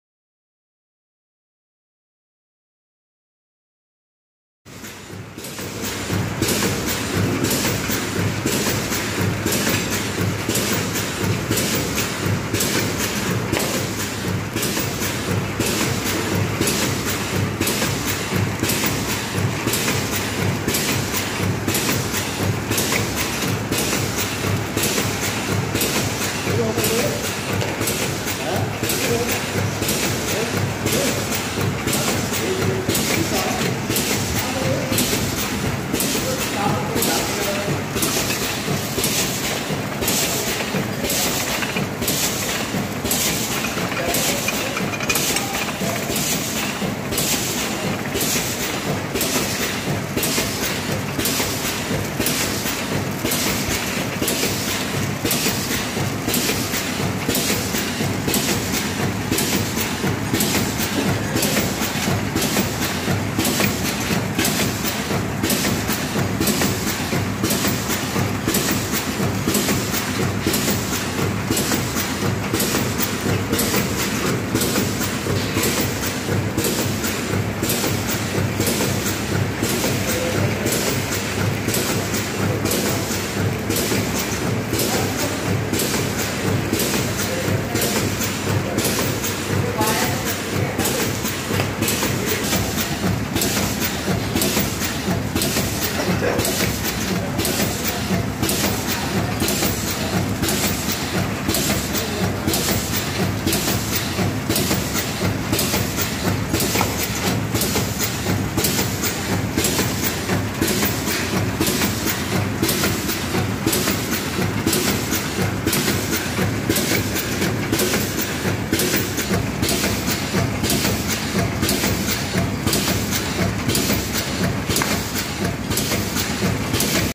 Tea packing machine
Suitable machine for packing granules like tea, rice, grains etc.
Packing range: 20gm to 250gm.